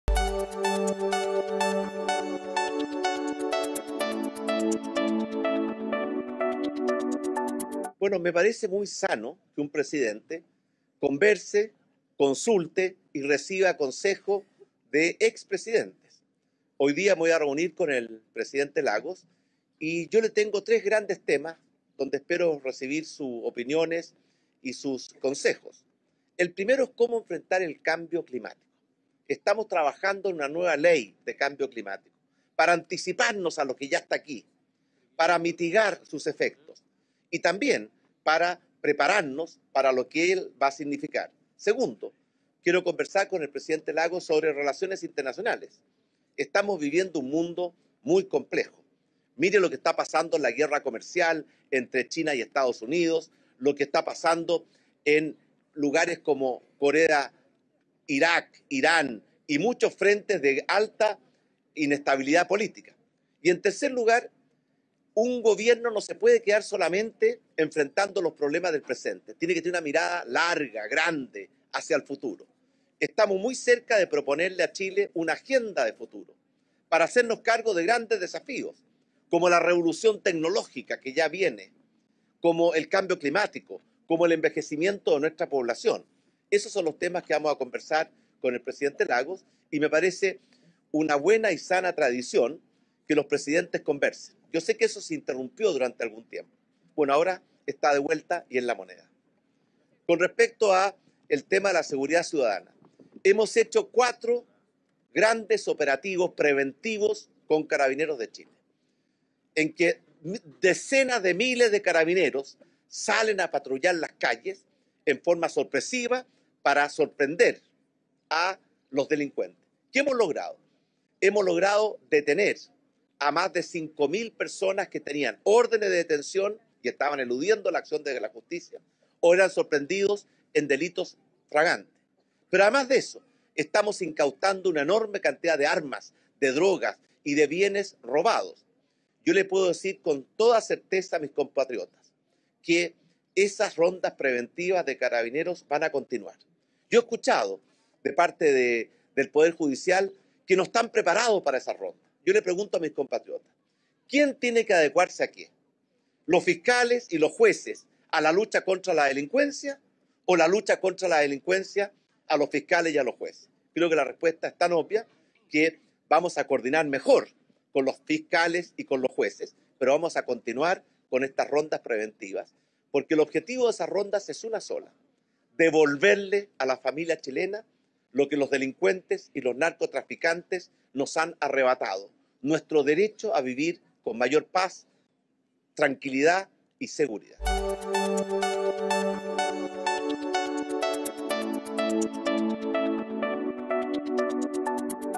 Ahora en DF: Piñera adelanta los temas a tratar en reunión con ex presidente Lagos
El mandatario Sebastián Piñera adelantó, en horas de la mañana, los temas que tratará en su reunión con el ex presidente Ricardo Lagos y destacó que lo más importante será una nueva ley de cambio climático en la que se está trabajando.